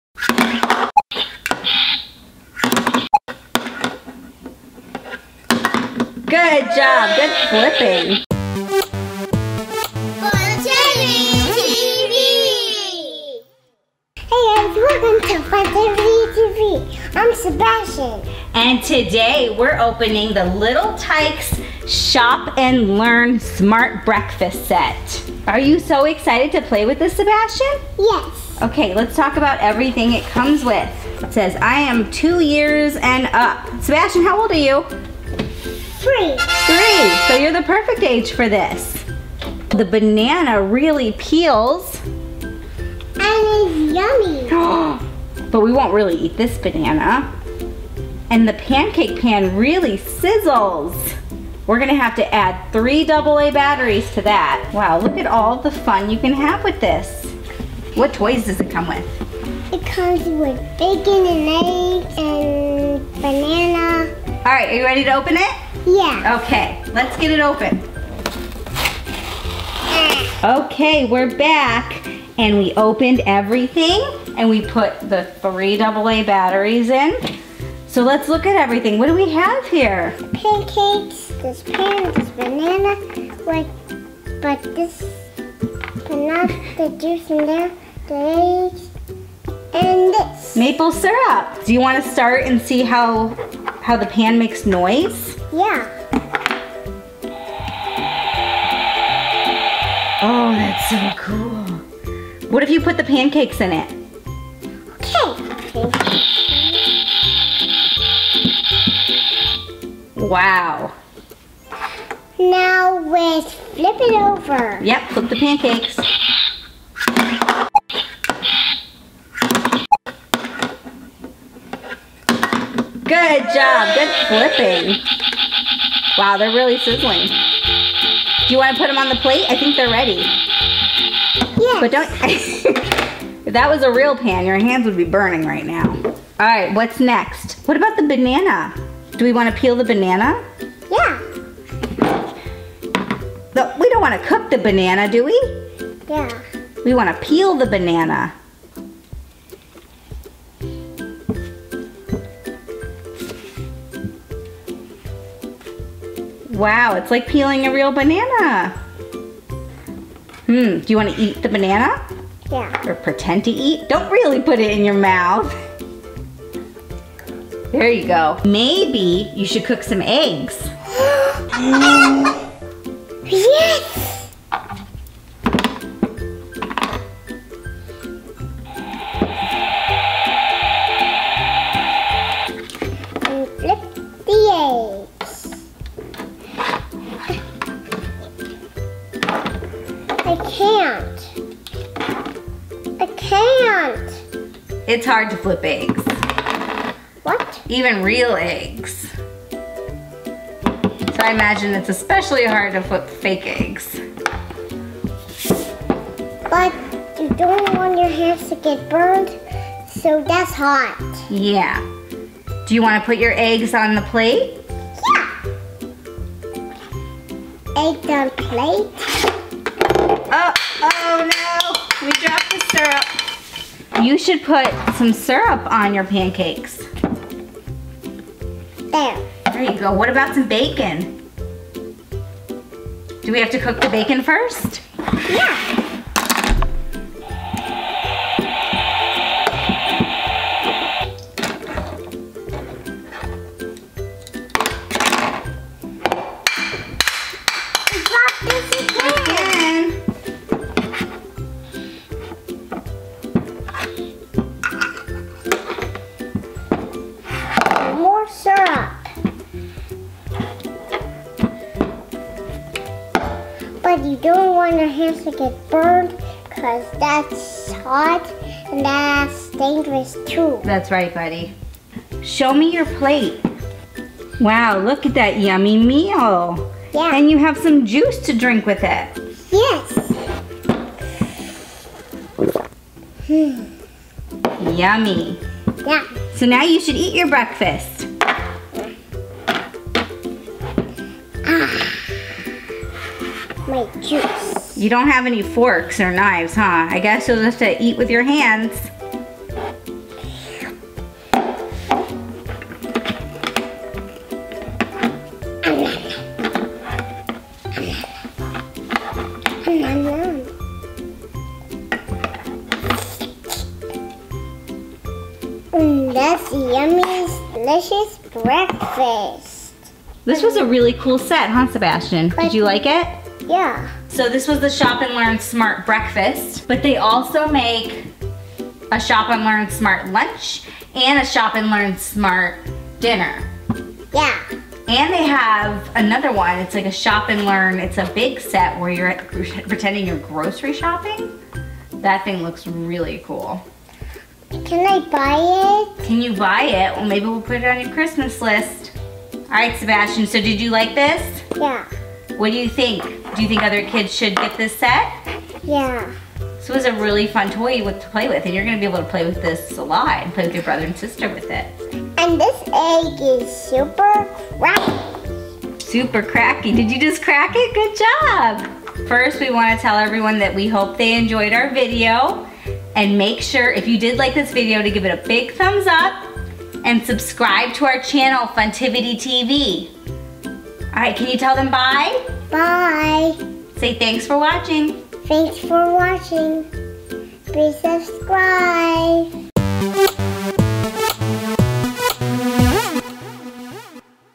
Little Tikes Shop 'N Learn Smart Breakfast - Funtivity TV Toy Review and Play
Three year old Sebastian had so much fun with this toy review! He got to play with the Little Tikes Shop 'N Learn Smart Breakfast. The banana really peels and the eggs crack and sizzle! This is such a fun toy for small children.